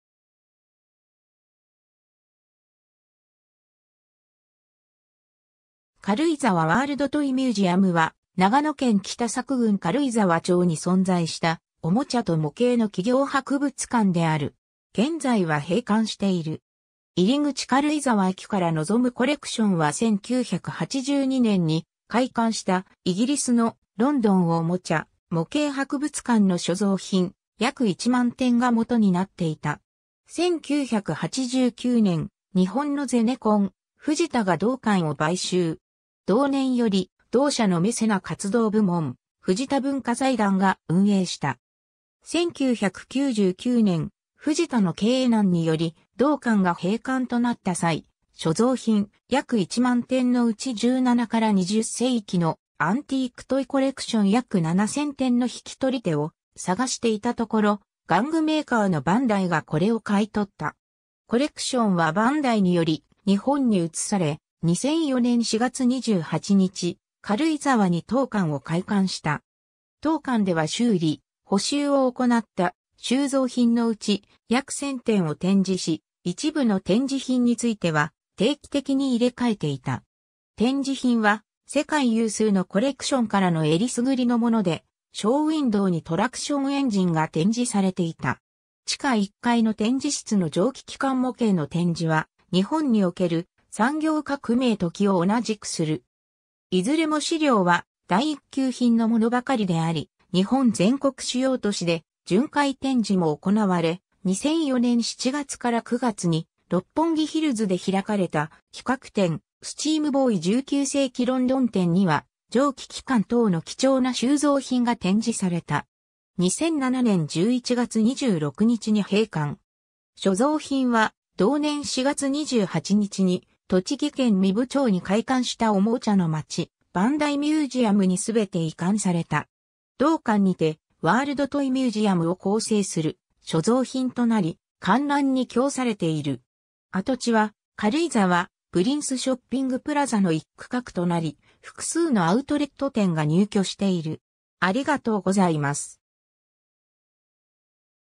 軽井沢ワールドトイミュージアム
日本の企業博物館_(廃止)
長野県の博物館_(廃止)
軽井沢ワールドトイミュージアム（かるいざわワールドトイミュージアム）は長野県北佐久郡軽井沢町に存在した、おもちゃと模型の企業博物館である。
現在は閉館している。
 入口 軽井沢駅から望む コレクションは1982年に開館したイギリスの「ロンドンおもちゃ・模型博物館」の所蔵品約10,000点が基になっていた。
1989年、日本のゼネコン、フジタが同館を買収。
同年より同社のメセナ活動部門「藤田文化財団」（後、藤田トイミュージアム財団）が運営した。
1999年、フジタの経営難により同館が閉館となった際、所蔵品約10,000点のうち17〜20世紀のアンティーク・トイコレクション約7,000点の引き取り手を探していたところ、玩具メーカーのバンダイがこれを買い取った。
コレクションはバンダイにより日本に移され、2004年4月28日、軽井沢に当館を開館した。
 当館では修理・補修を行った収蔵品のうち約1,000点を展示し、一部の展示品については定期的に入れ替えていた。
展示品は世界有数のコレクションからの選りすぐりのものでショウウインドウにトラクションエンジンが展示されていた。
 地下一階の展示室の蒸気機関模型の展示は日本における産業革命と期を同じくする。
いずれも資料は第一級品のものばかりであり、日本全国主要都市で巡回展示も行われ、2004年7月から9月に六本木ヒルズで開かれた企画展「スチームボーイ・19世紀ロンドン展」には、蒸気機関等の貴重な収蔵品が展示された。
 2007年11月26日に閉館。
所蔵品は同年4月28日に栃木県壬生町に開館したおもちゃのまちバンダイミュージアムにすべて移管された。
同館にて「ワールドトイミュージアム」を構成する所蔵品となり、観覧に供されている。
 跡地は軽井沢・プリンスショッピングプラザの一区画となり、複数のアウトレット店が入居している。